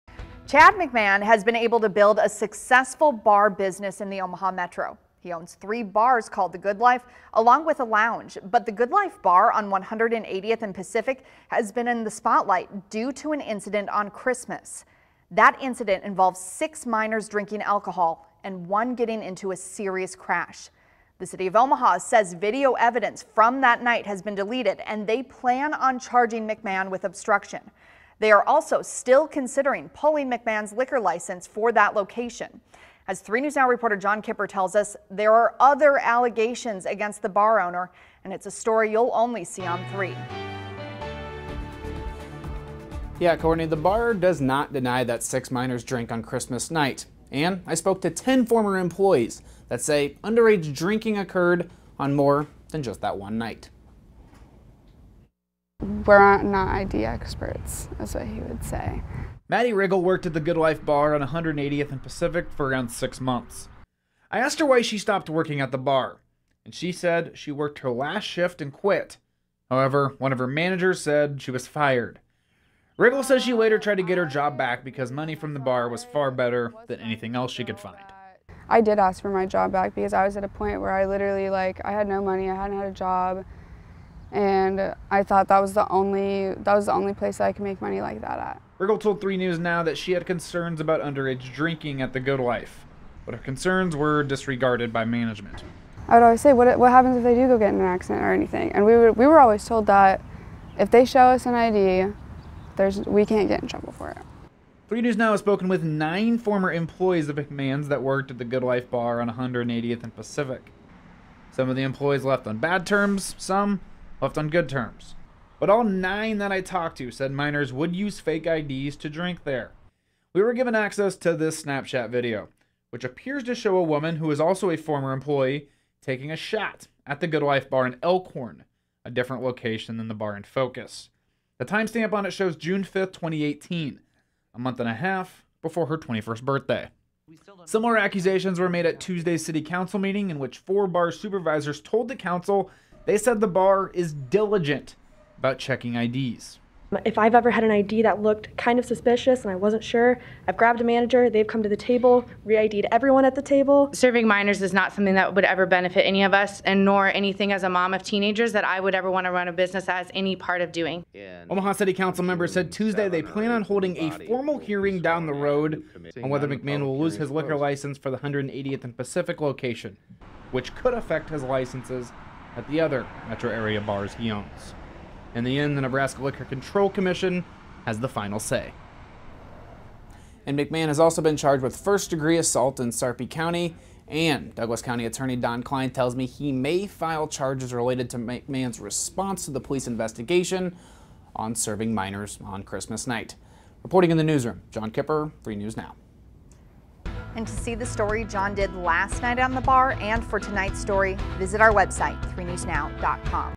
Former employees say underage drinking occurred regularly at Good Life bar
3 News Now has spoken with nearly a dozen former employees of The Good Life bar in West Omaha. Current employees told the city council yesterday that they are diligent about checking IDs.